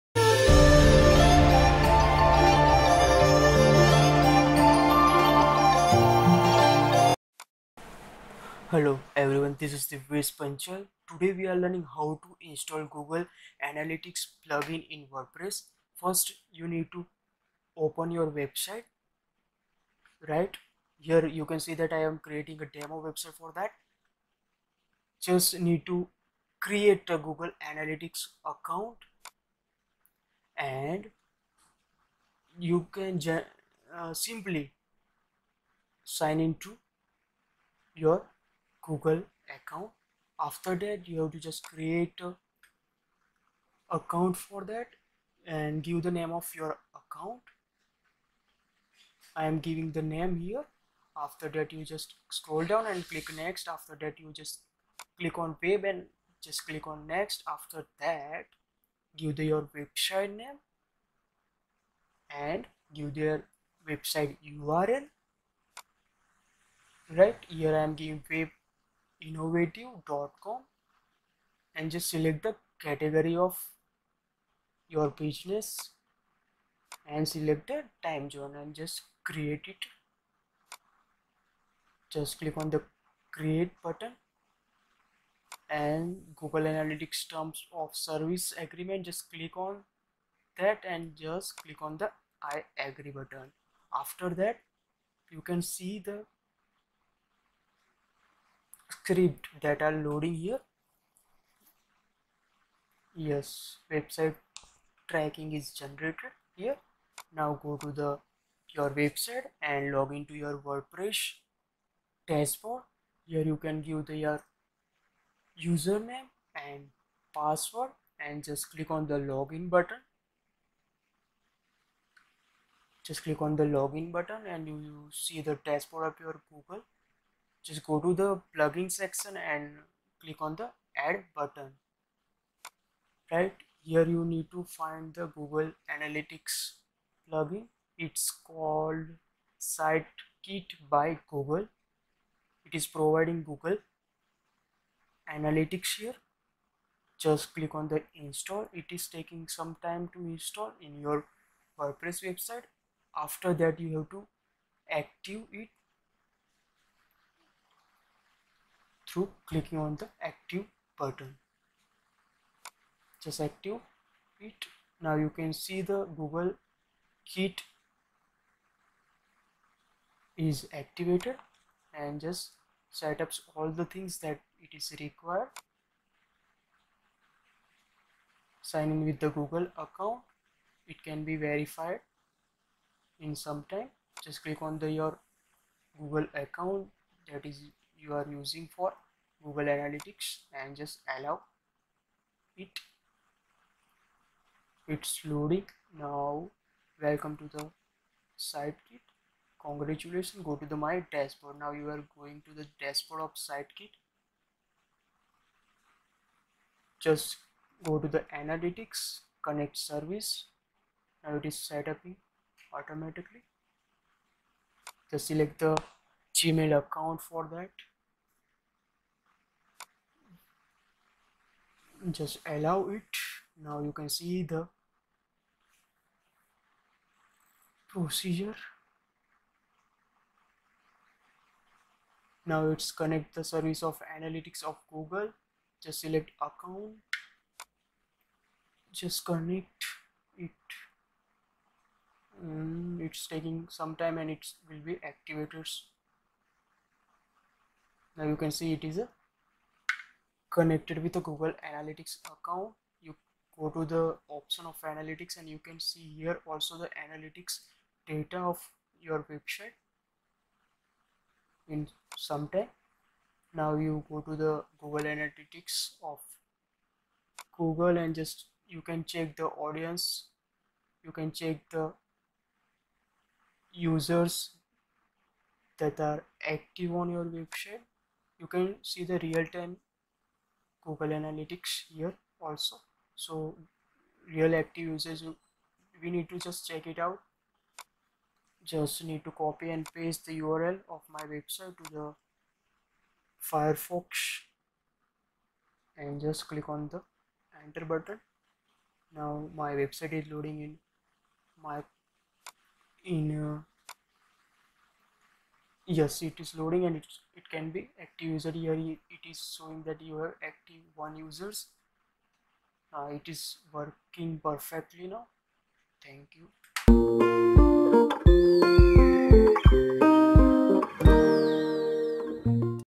How To Install Google Analytics On WordPress - Tutorial for Beginners 2020!
Learn How to Easily Install Google Analytics to your WordPress Website, Blog, or eCommerce website in 2020.

Google Analytics is used to track website activity such as session duration, pages per session, bounce rate etc. of individuals using the site, along with the information on the source of the traffic.

Summary of this Tutorial:

Step 1: Create a google analytics account

step 2: Install site kit by google analytics plugin
step 3: Setup google analytics